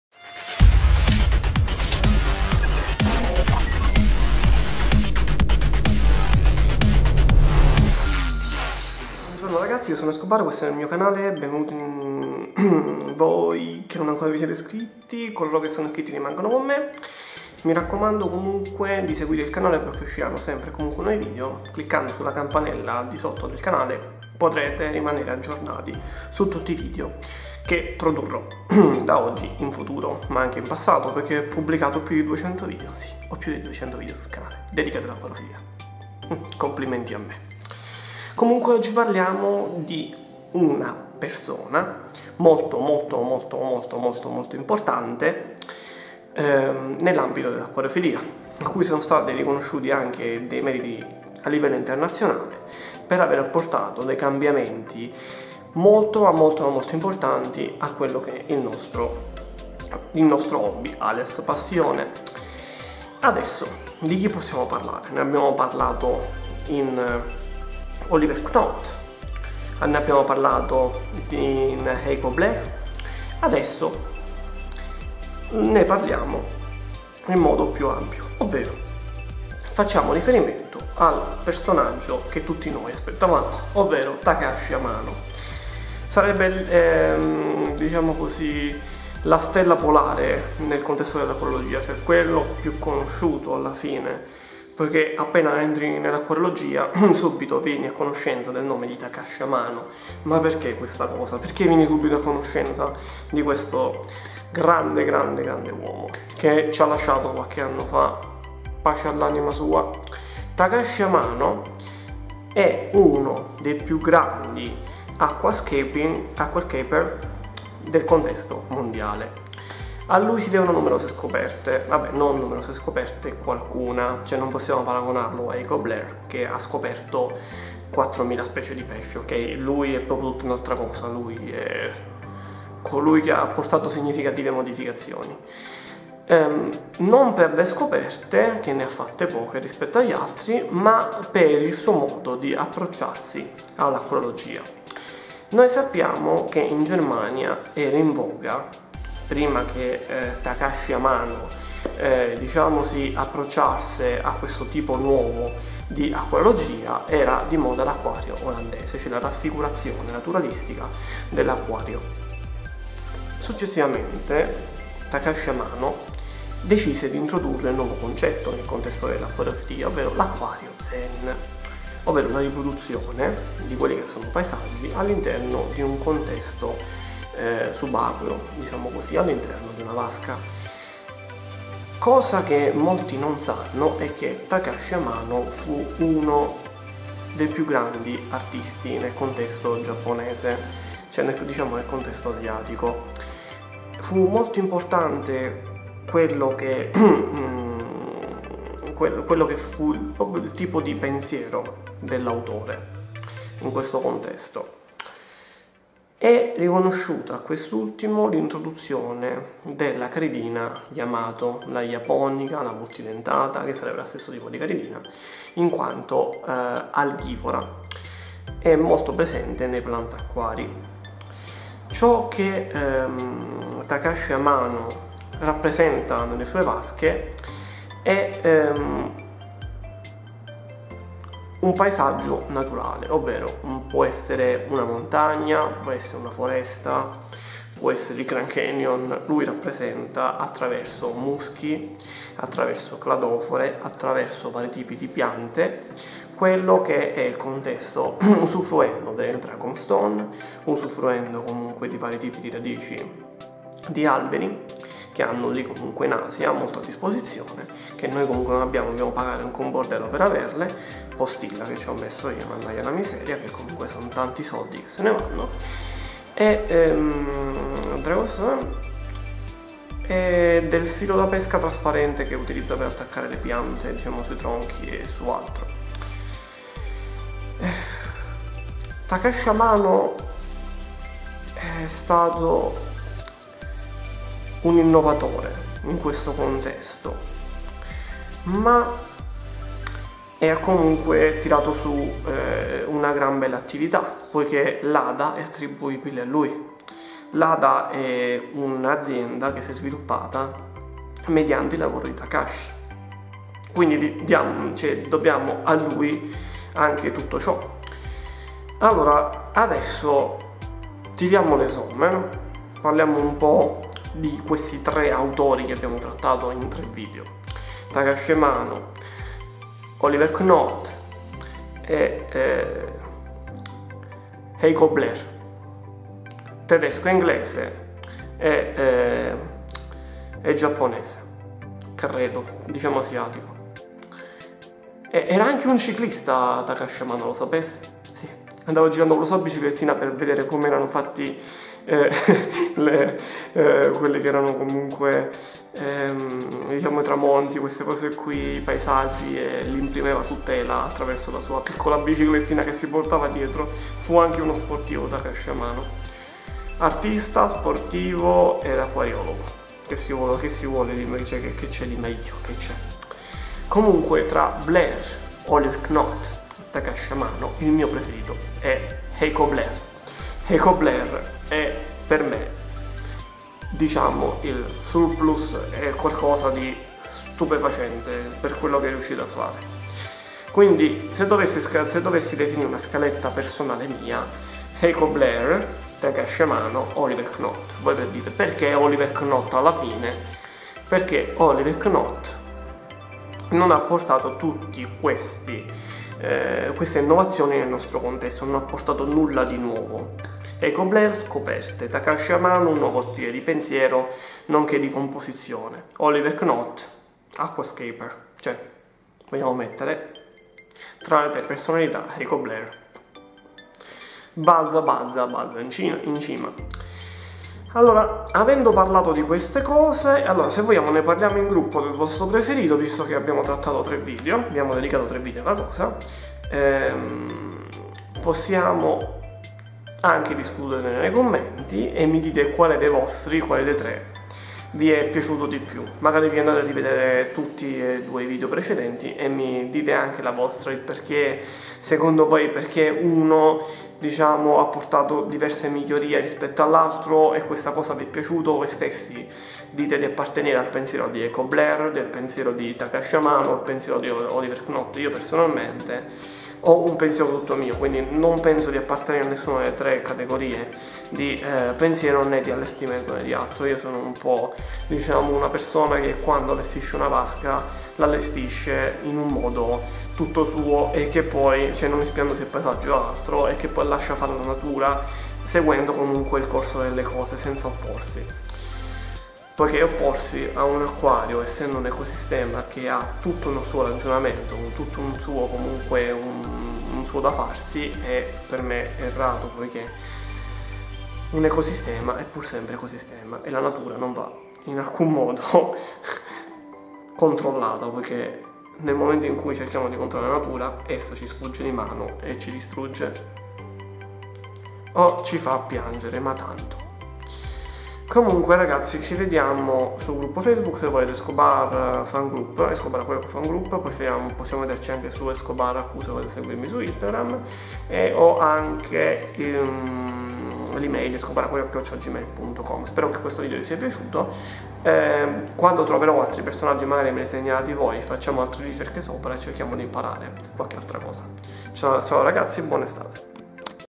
Acquari e la loro storia TAKASHI AMANO artista o acquariofilo? (capitolo 3)
gruppo facebook:
Istagram:
escobaraq